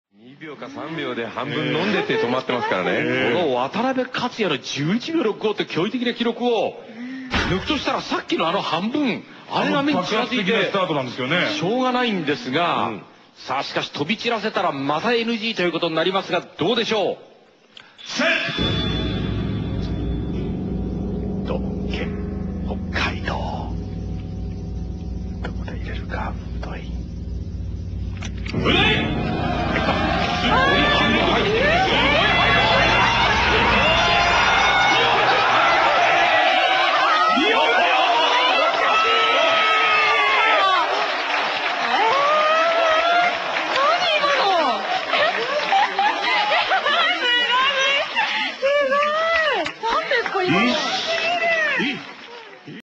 Can you drink any faster?
Japanese game show drink a 2 ltrs bottle in lighting speed